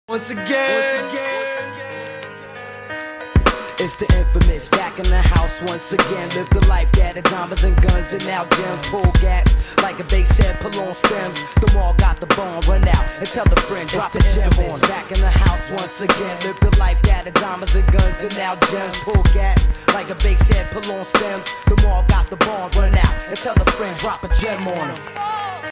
Elijah Pyle Mobb Deep Snowboard Edit
Filmed at Mt. Bachelor, Oregon

Thanks to:
Burton Snowboards
Anon Optics
Mt. Bachelor
Woodward
Tactics
Blackstrap